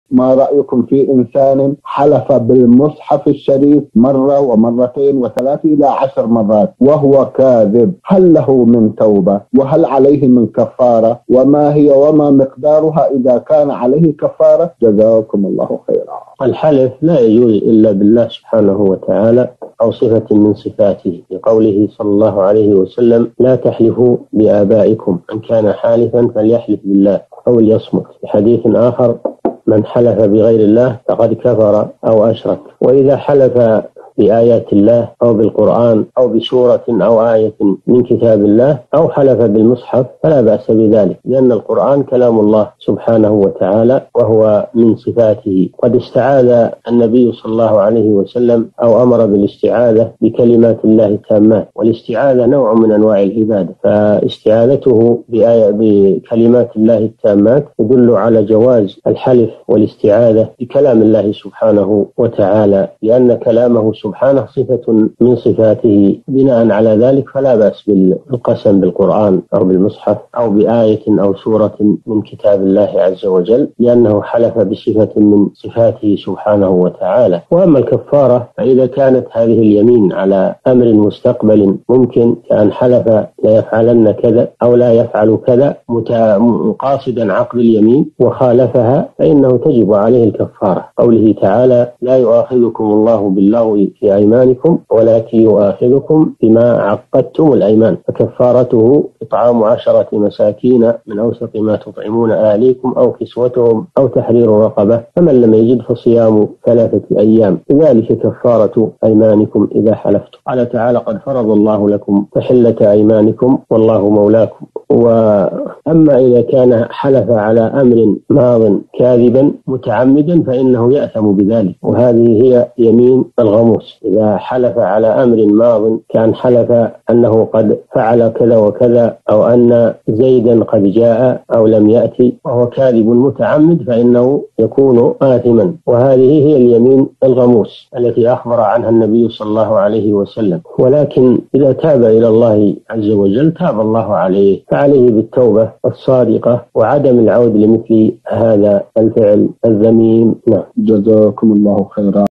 ماحكم من حلف بالمصحف الشريف مرات عديدة وهو كاذب هل له توبة وهل عليه كفارة وماهي وما مقدارها؟ الفوزان
نص السؤال:
ما رأيكم في إنسان حلف بالمصحف الشريف مرة ومرتين وثلاث إلى عشر مرات وهو كاذب هل له من توبه وهل عليه من كفارة وما هي وما مقدارها إذا كان عليه كفارة جزاكم الله خيراً؟
السيرة الذاتية :
للاشتراك في قناة الشيخ صالح الفوزان

رئيس الفريق
لمشروع كبار العلماء